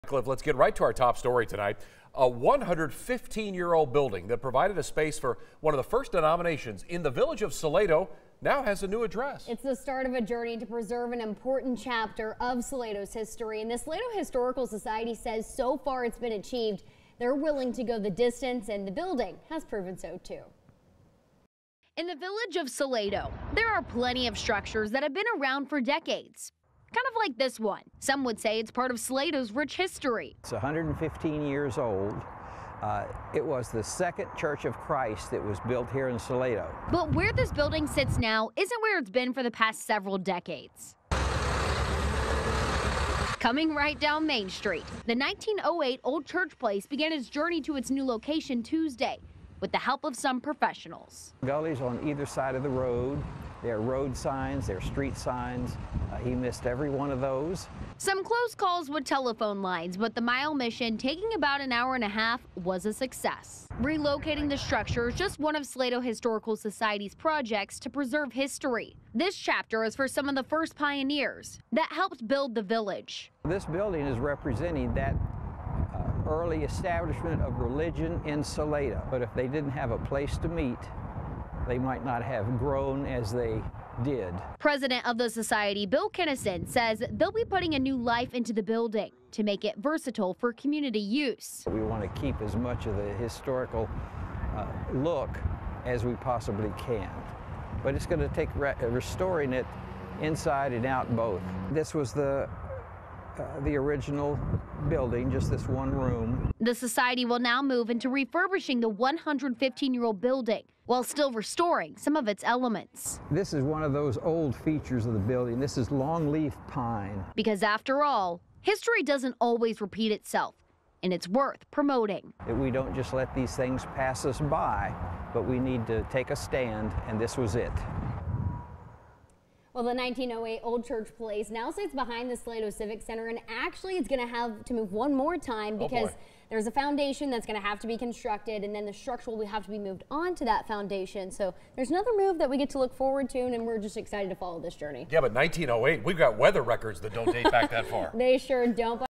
A successful journey in preserving history | Historic Salado structure moved a mile to new address
The historic 1908 Old Church Place, which is 115-years-old, was successfully moved to a new location Tuesday.